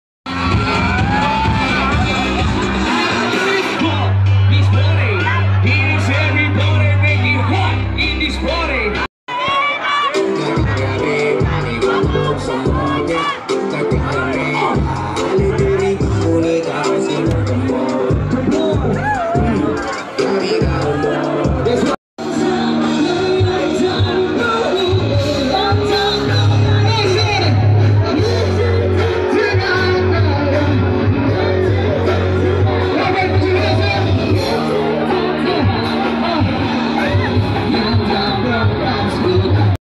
Foam party 2022 | Jen kulit vlogs
Foam party 2022 in talipapa bayawan city negros oriental.